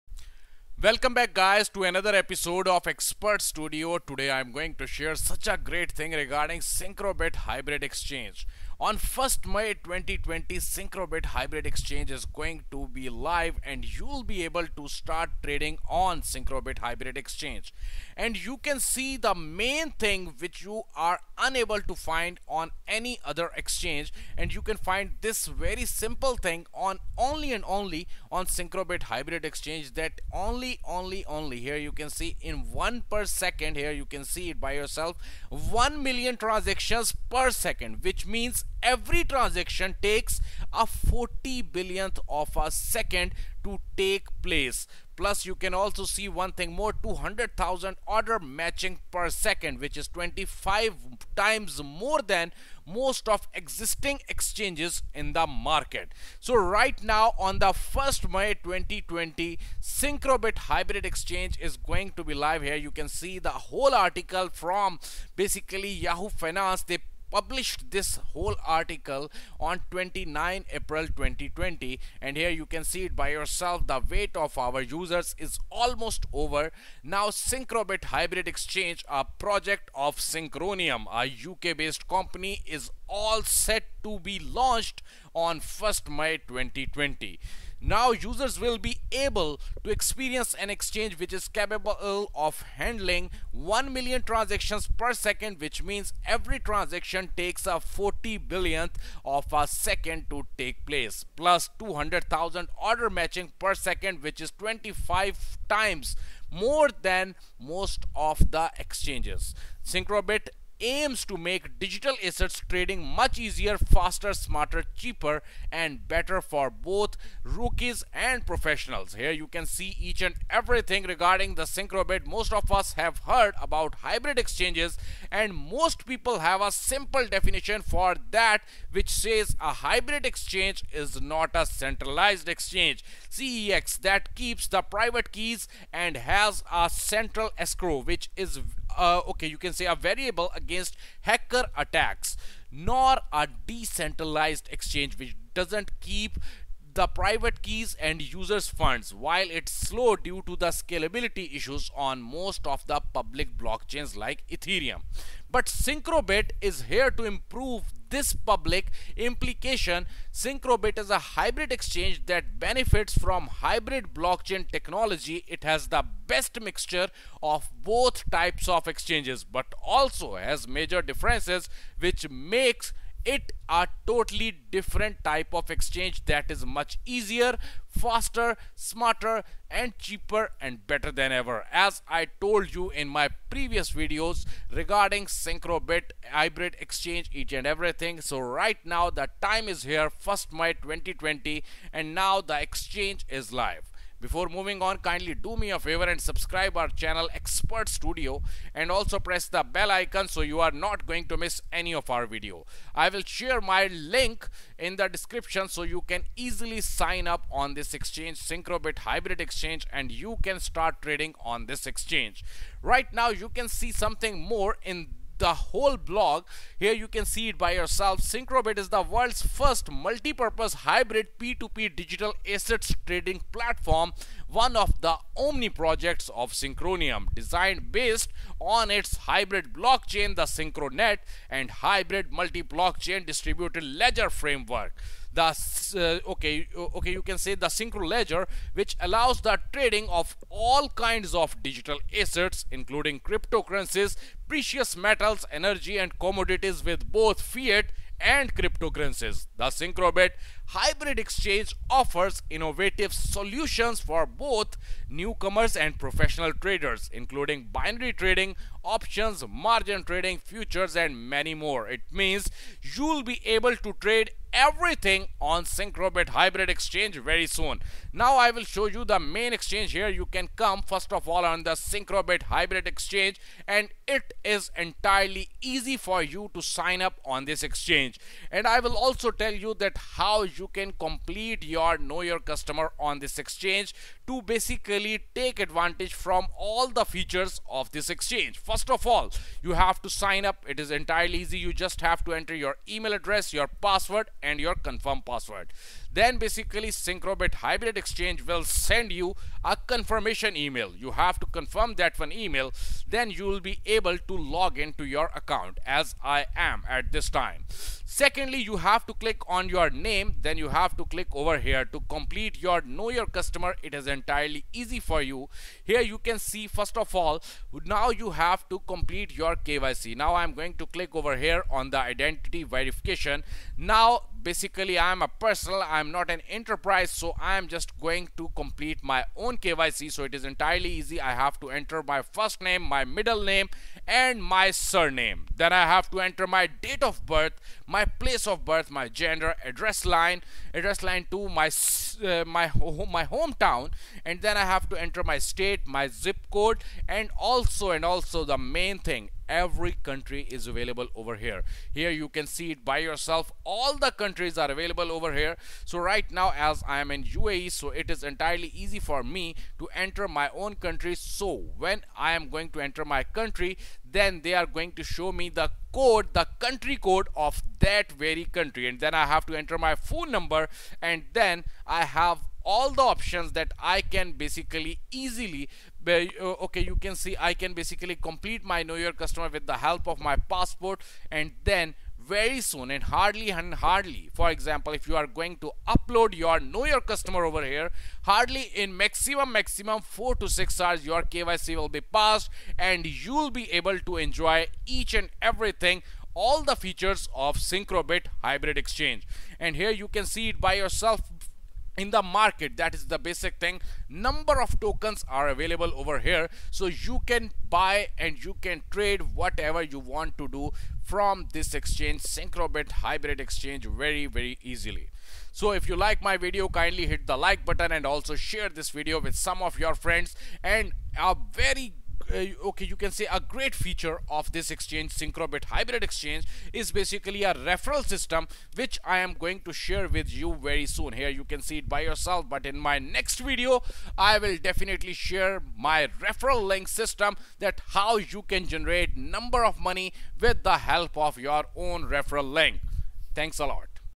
Synchrobit Hybrid Exchange Live On 1st May 2020|Best Things To Know
SynchroBit is the world's first Multipurpose Hybrid P2P Digital Assets Trading Platform, one of the Omni Projects of SYNCHRONIUM designed based on its hybrid blockchain, the SynchroNet.

Synchrobit Hybrid
Exchange Live On 1st May 2020
Best Things To Know

What Is Synchrobit Exchange?
How Synchrobit Exchange Will Work?